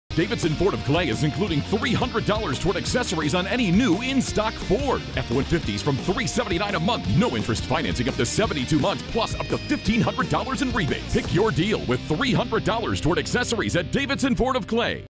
Trick your ride at Davidson Ford of Clay this month!
This month Davidson Ford is including $300 towards accessories of your choice on any new Ford in stock! Lease a new 2017 Ford F-150 from only $379/ month. No interest financing 0% for 72 months PLUS up to $1,500 in rebates. Pick your deal with $300 towards accessories at Davidson Ford.

Nice People to do business with!